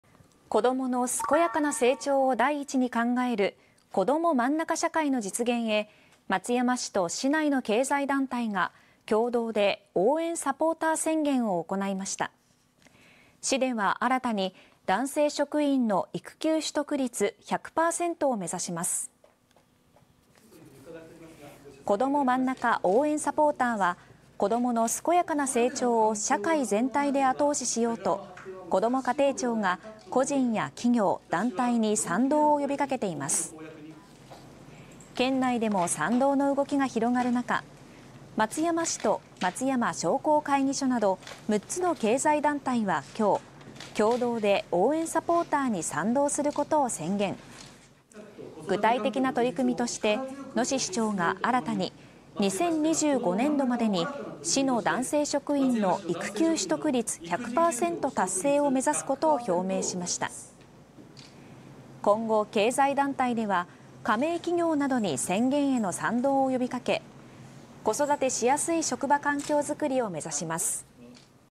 「こどもまんなか応援サポーター」松山市や経済団体も宣言 市職員育休１００％目指す【愛媛】 (23/07/28 18:50)
子どもの健やかな成長を第一に考える「こどもまんなか社会」の実現へ、松山市と市内の経済団体が２８日に共同で 応援サポーター 宣言しました。市は 男性職員 の 育休 取得率１００％を目指します。

「こどもまんなか応援サポーター」は、子どもの健やかな成長を社会全体で後押ししようとこども家庭庁が個人や企業・団体に賛同を呼びかけています。

この呼びかけに松山市と松山商工会議所など６つの経済団体が、共同で応援サポーターに賛同することを宣言。具体的な取り組みとして、野志市長が新たに２０２５年度までに、市の男性職員の育休取得率１００％達成を目指すことを表明しました。

経済団体は加盟している企業などにも賛同を呼びかけ、子育てしやすい職場環境づくりを目指します。